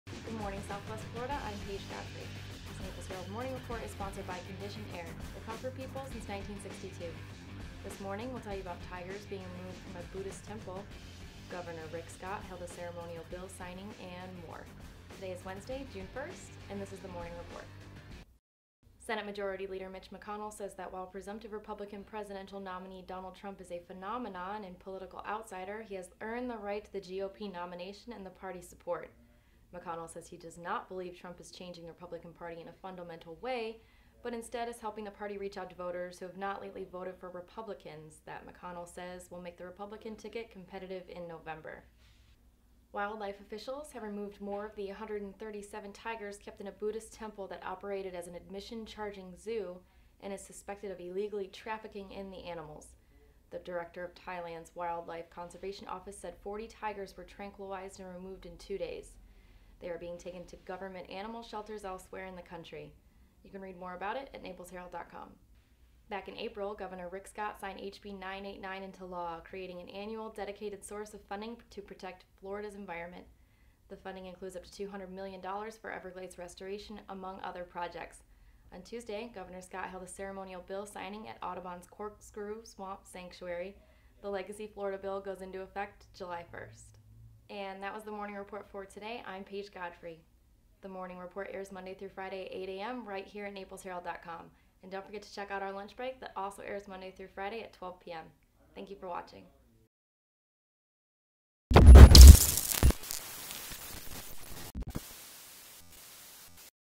06-01-16 Naples Herald Morning Report
This is the June 1st Morning Report. This morning we'll tell you about tigers being removed from a Buddhist temple, Govenor Rick Scott held a ceremonial bill signing and more...